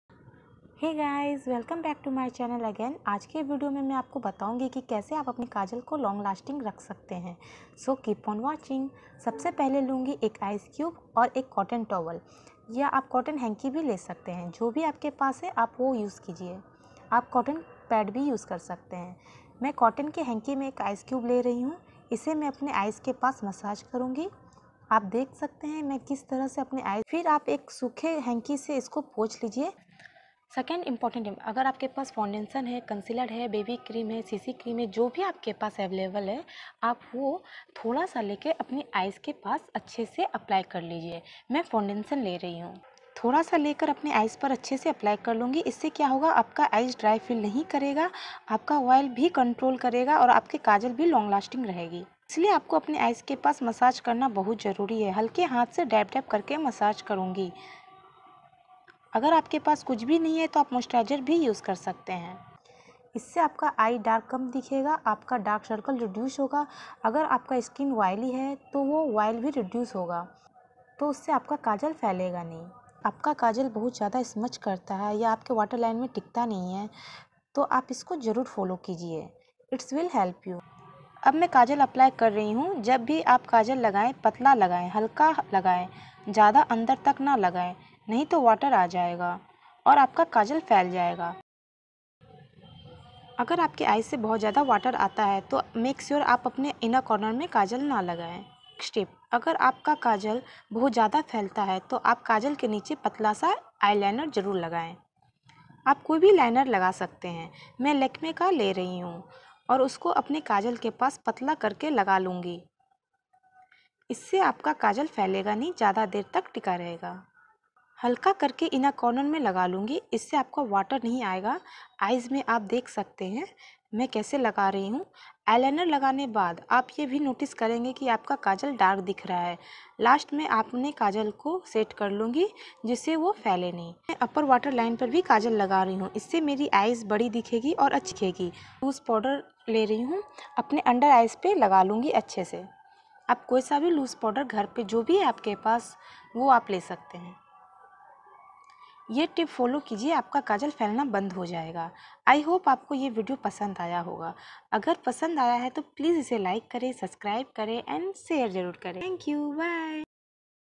Bold Kajal Look with tips & trick | How to Make your Kajal long lasting & smudge proof
How to make kajal and put it to long lasting & smudge proof. please look at the video and see the bold kajol makeup tutorial.

Follow Me --